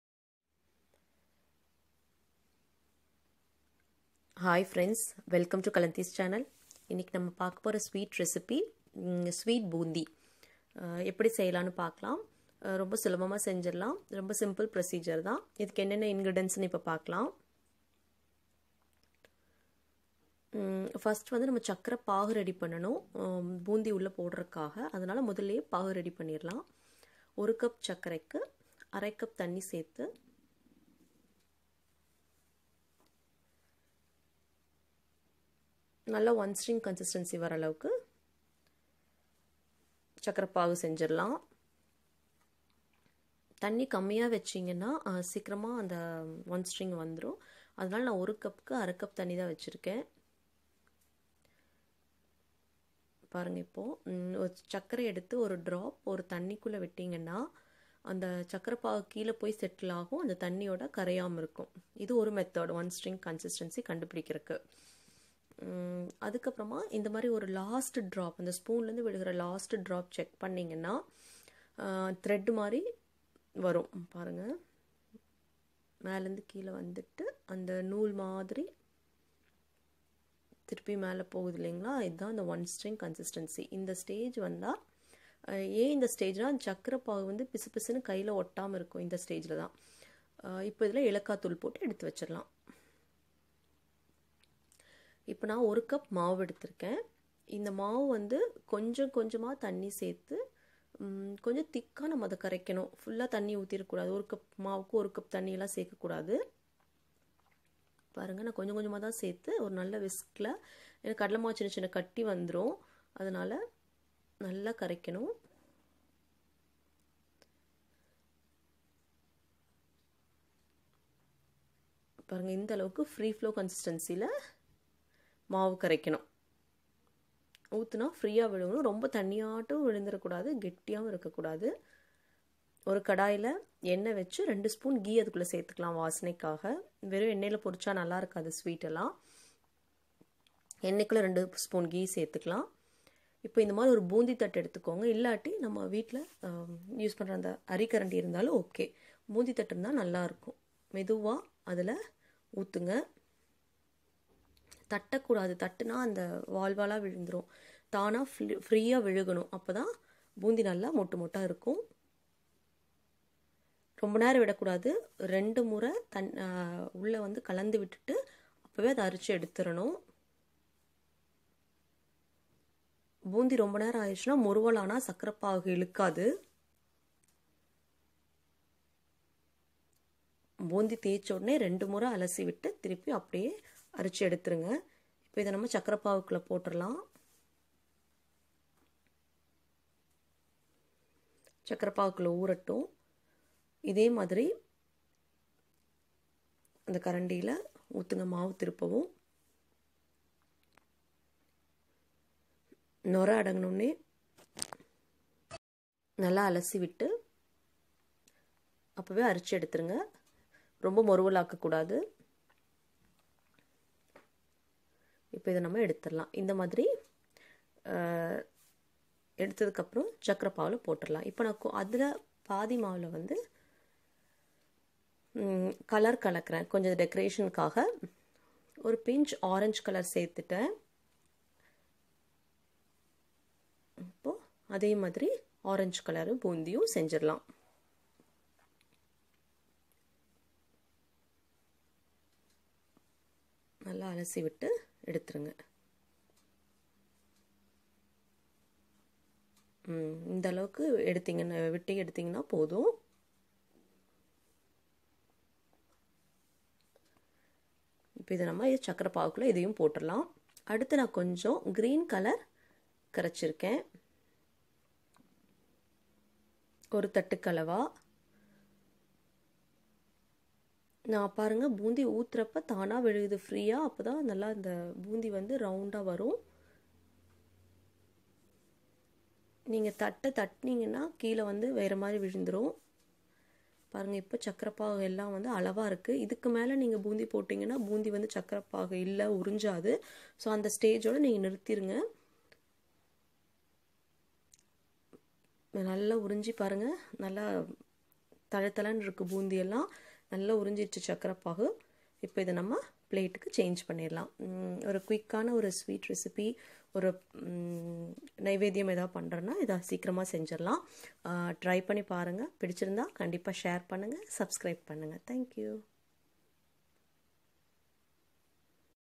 Sweet Boondi Recipe in Tamil/Diwali Sweet/ Sweet Boondi Recipe/Deepavali Sweet
In this video we will show you how to make Sweet Boondi Recipe very easily in home. in this video I have shown you how to make one string consistency for sugar. Try this Recipe and post your feedback in the comment section below..